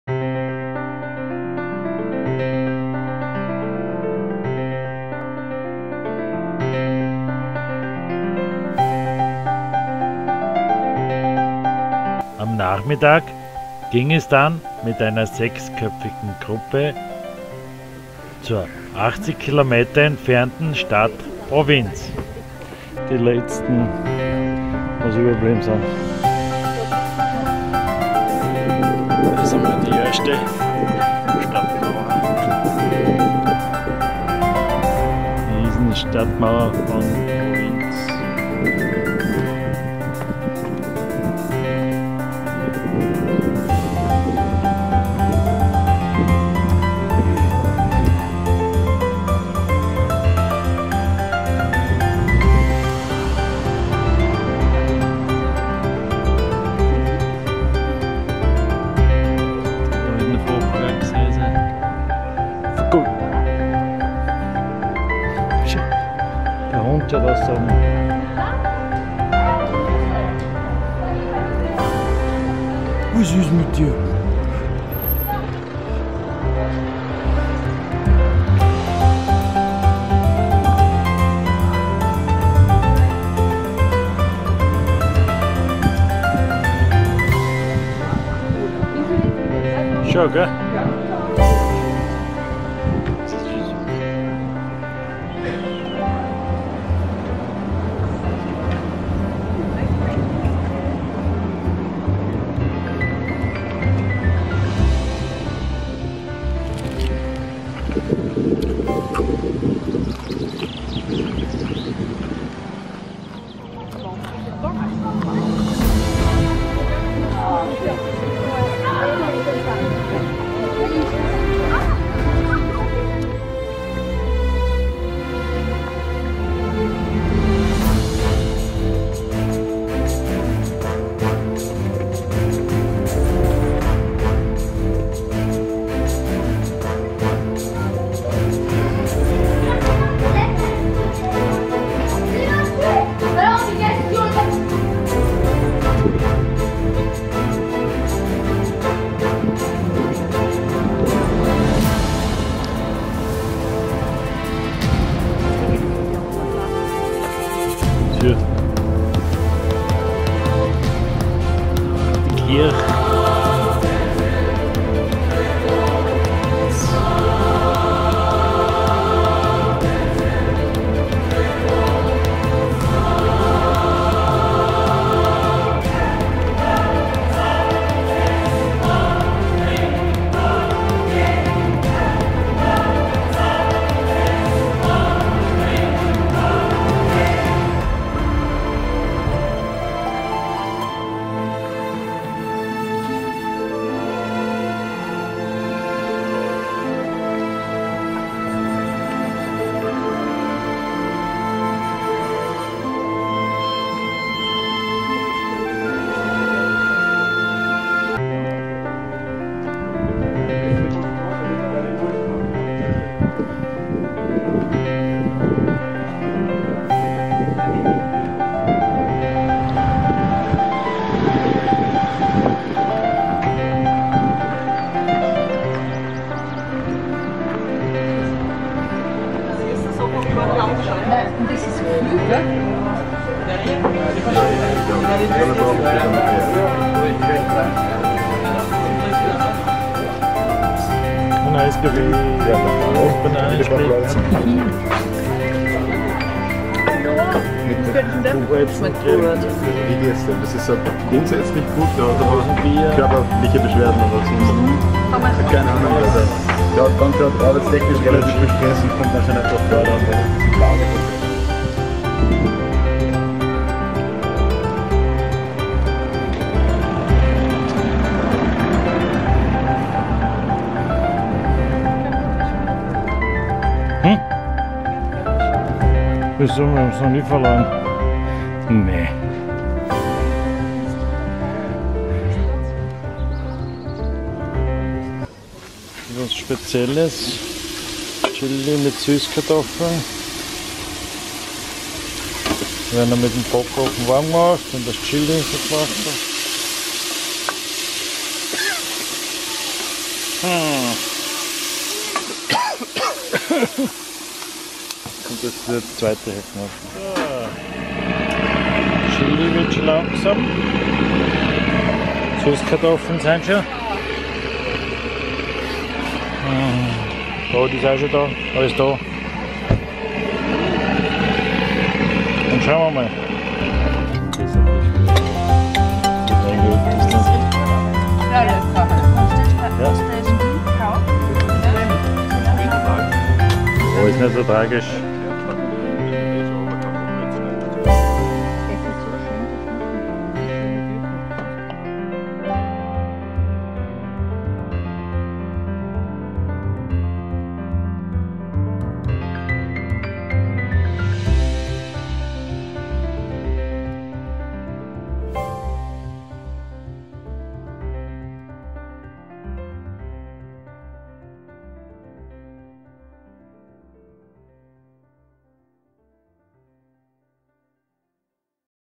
Visit the medieval city "Provins" - Climb & Fun V
Am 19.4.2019 fuhren wir im Rahmen unseres Climb & fun Events ins
ca. 80 km  Provins. Nach einer 1 ½ stündigen Fahrt kamen wir in Provins an und fanden dann auch gleich bei der Stadtmauer einen gratis Parkplatz, direkt bei der Tourismusinfo. Dort holten wir uns dann noch gleich eine Karte vom Ort und ab ging es in die Oberstadt von Provins.  Provins wurde zum ersten Mal 802
urkundlich erwähnt und war früher eines der größten Märkte des Landes. Sehenswert ist die Stadtmauer, die 1226 erbaut wurde und 1200 Meter lang und mit 22 Türmen bestückt ist, die mittelalterliche Oberstadt mit vielen Fachwerkhäusern, die Kirche Saint-Quiriace aus dem 12. Jhd. Und die wirklich imposante Burg Tour Cesar, ebenfalls aus dem 12. Jhd. Die Burg ist vermutlich einzigartig in ihrem Aussehen und wirklich gut erhalten. Im Ort gönnten wir uns noch eine kleine Jause und einem Eiskaffee und dann ging es auch schon
wieder zum Bus und wir fuhren – kurz noch zum Lidl und dann - die 50 km wieder zu unserem Camp